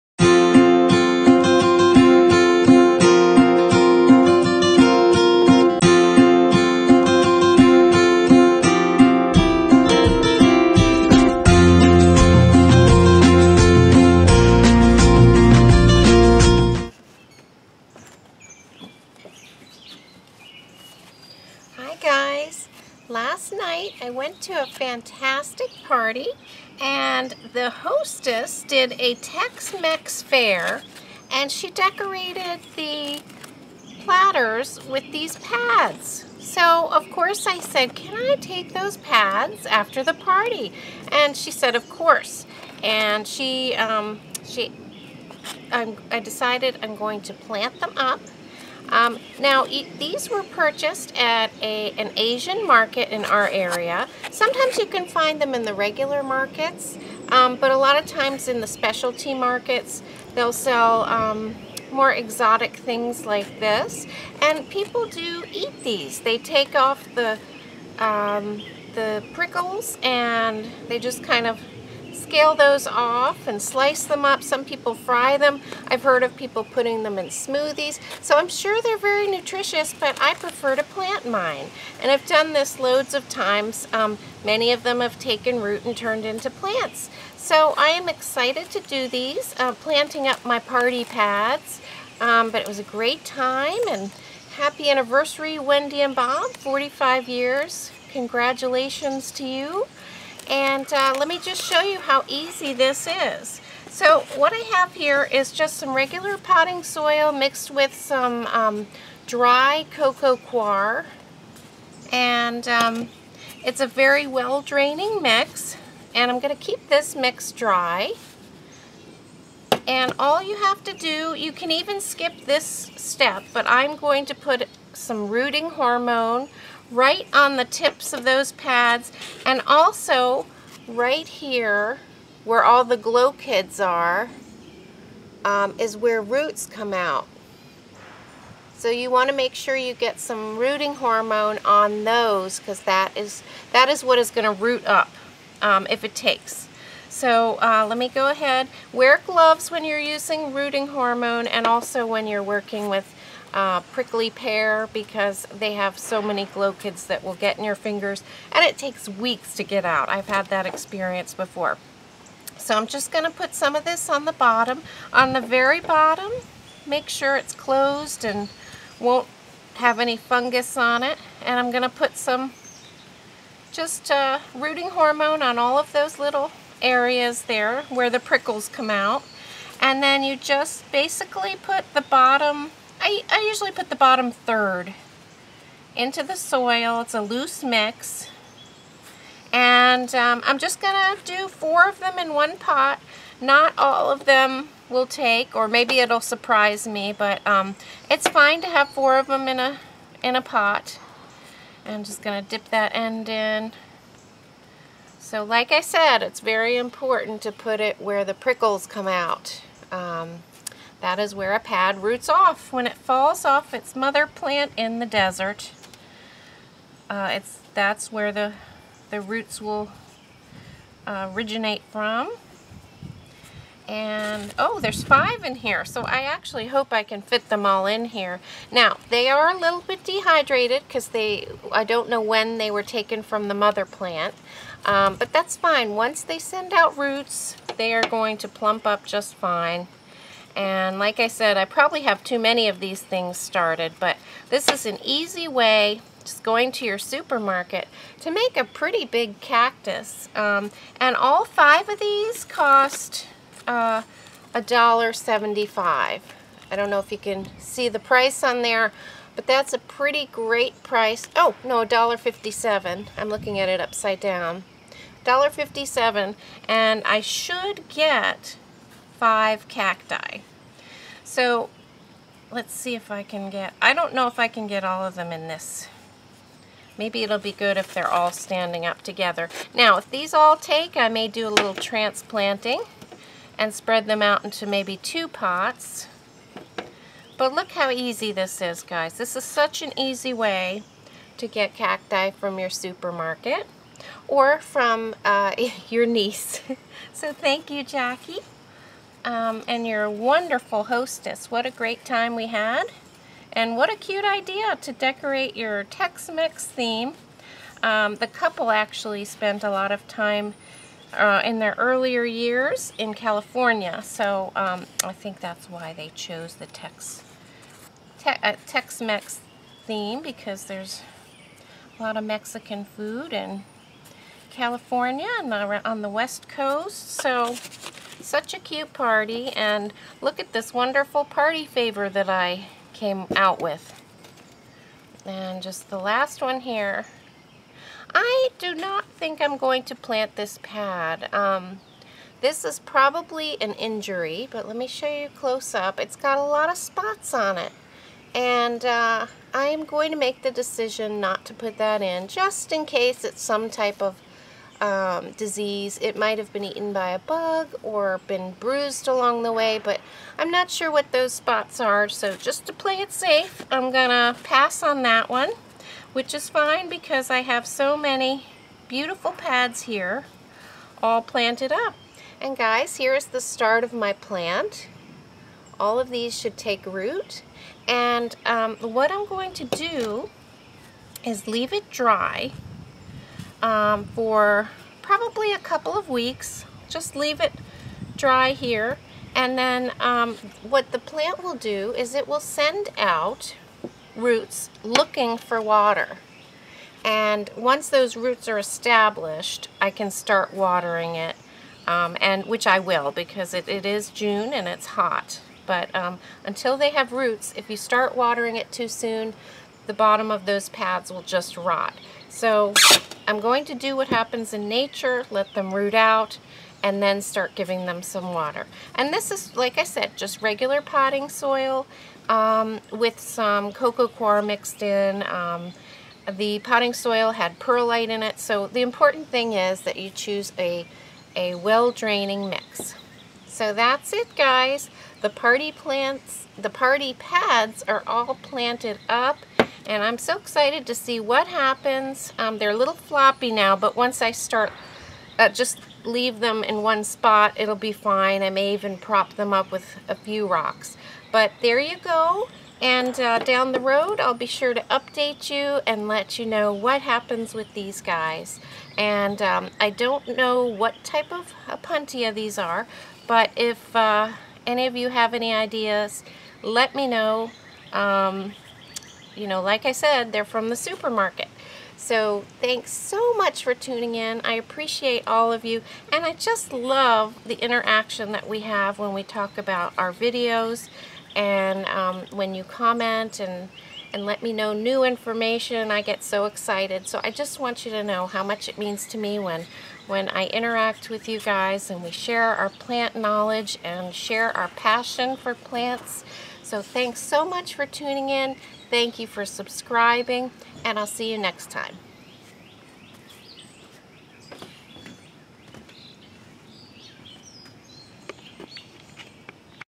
Planting Party Pads
I brought these Opuntia pads home from a Tex-Mex party, and I planted them up.  Pads can often be purchased at specialty grocery stores, it's a great way to get an inexpensive plant!